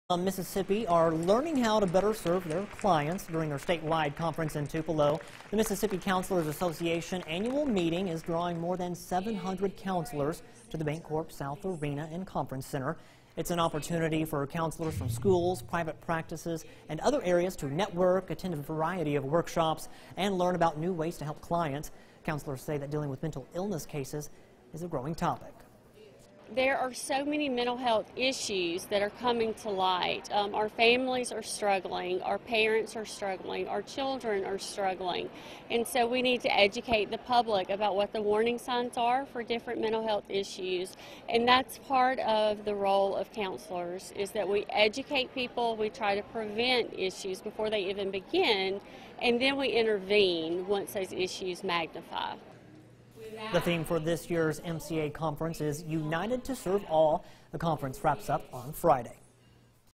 Counseling Association
Counselors from Mississippi are learning how to better serve their clients, during their statewide conference in Tupelo.